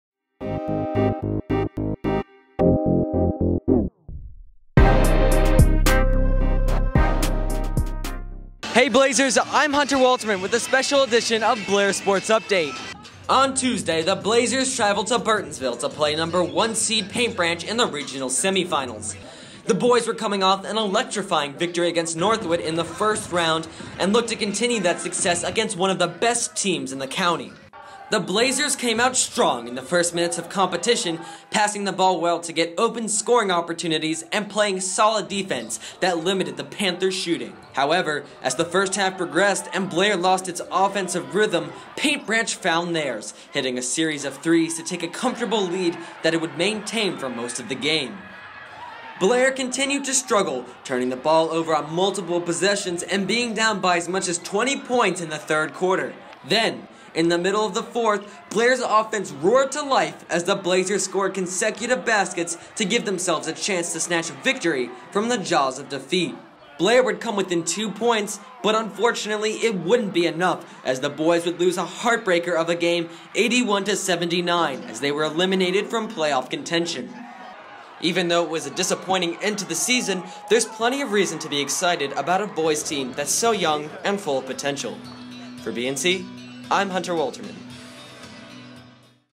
Blair vs. Paint Branch Boys Basketball Playoff Recap
This is a recap of the Blair vs. Paint Branch boys basketball playoff game.